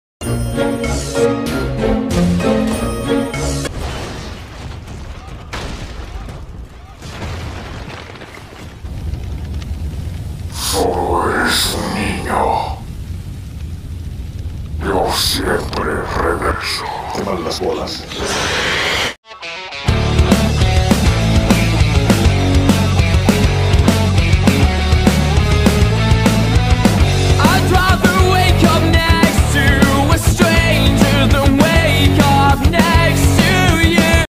Springtrap se quema🔥....otra vez| FNAF security breach parodia (SZ)
en vez de yo siempre regreso sería, yo siempre me quemó :v